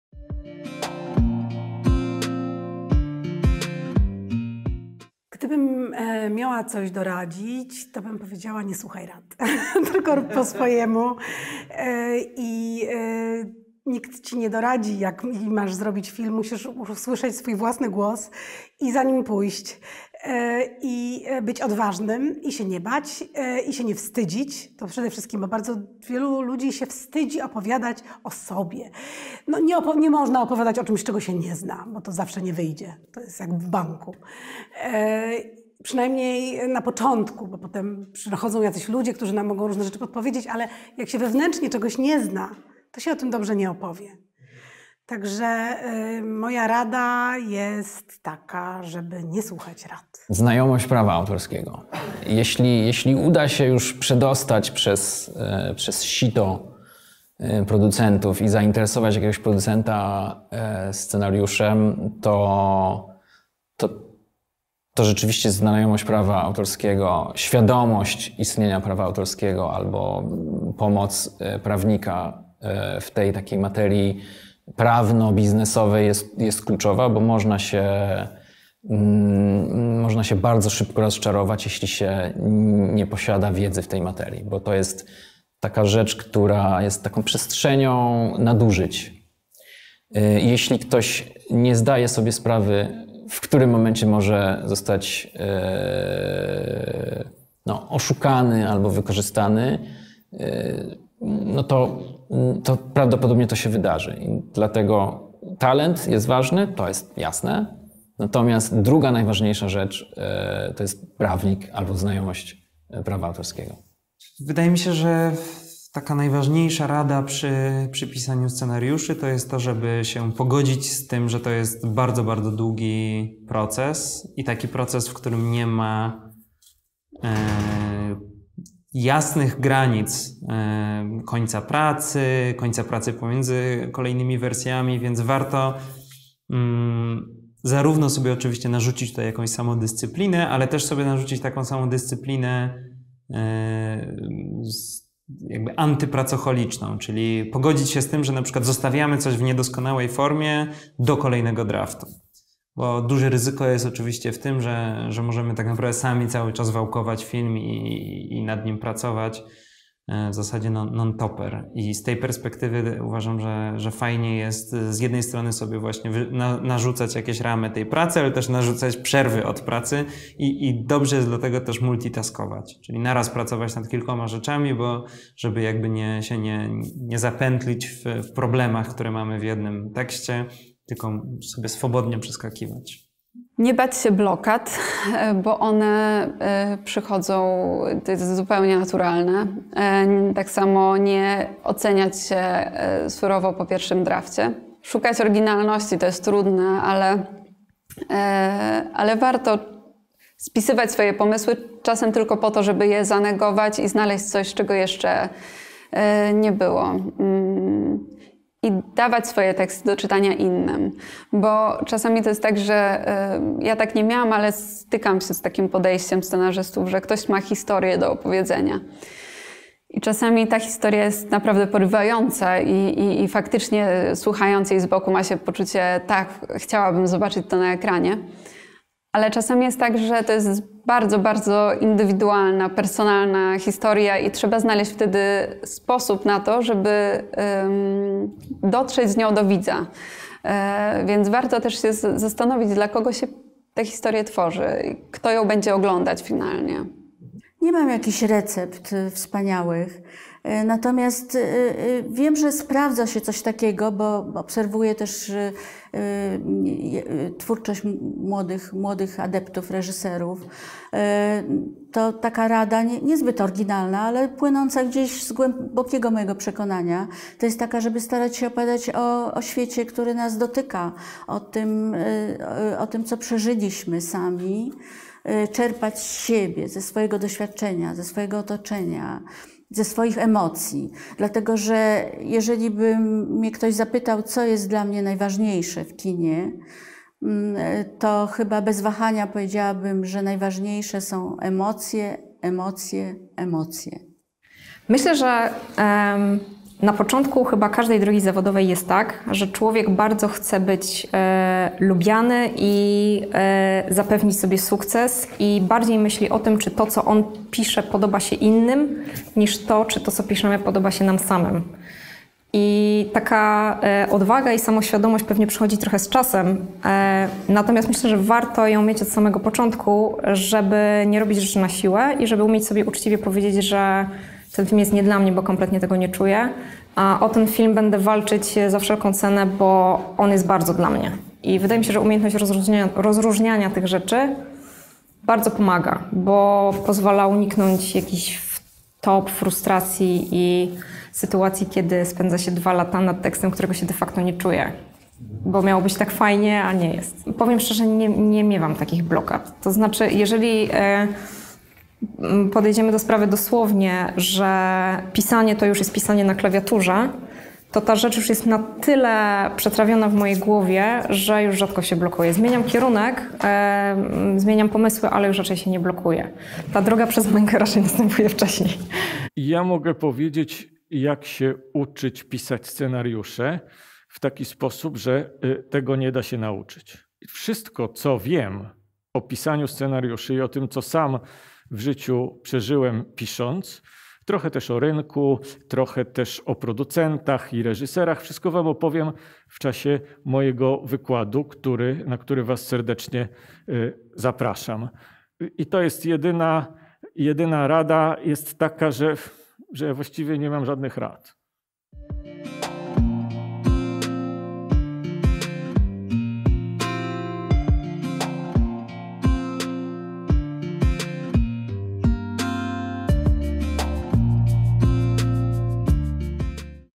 Jak pracują scenarzyści? | Odcinek 6 | Rady
Jak pracują scenarzyści? Praktyczne wskazówki w sześciu aktach

Topowi scenarzyści, konsultanci scenariuszowi i piszący reżyserzy, czyli duża dawka merytorycznych wskazówek i wiedzy w sześciu aktach. Organizatorzy Script Fiesty zaprosili na wydarzenie, w ramach którego zaproszeni goście opowiedzieli o własnych doświadczeniach z pracy nad tekstem. Sześcioodcinkowy cykl miał swoją premierę online podczas Script Fiesty 9½.

Goście wydarzenia poruszali się po sześciu przestrzeniach tematycznych. Opowiedzieli o początkach pracy nad scenariuszem, swoich rytuałach twórczych i narzędziach, które pozwalają stawiać im kolejne kroki w procesie, poszukiwaniu inspiracji i tematu, o sposobie kreowania bohaterów, pisania dialogów i doborze odpowiedniej struktury narracji dla historii. Nie zabrakło także rozdziału poświęconego współpracy scenarzystów z konsultantami i producentami. Ostatni epizod stanowi zbiór dobrych praktyk, kluczowych rad dla przyszłych scenarzystów. Do udziału w wydarzeniu zaproszeni zostali m.in. Michał Oleszczyk (Pisarze. Serial na krótko), Mateusz Pacewicz (Boże ciało, Hejter), Kaja Krawczyk-Wnuk (Żeby nie było śladów, Polot), Łukasz M. Maciejewski (Król, Czerwony pająk), Anna Bielak (konsultacja: Magnezja), Krzysztof Rak (Bogowie, Sztuka kochania), Agnieszka Kruk (script coach), Maciej Ślesicki (Tato, Sara), Marta Szymanek (Wataha, Odwilż), Kinga Dębska (Moje córki krowy, Plan B) i Magdalena Łazarkiewicz (Na koniec świata, Powrót).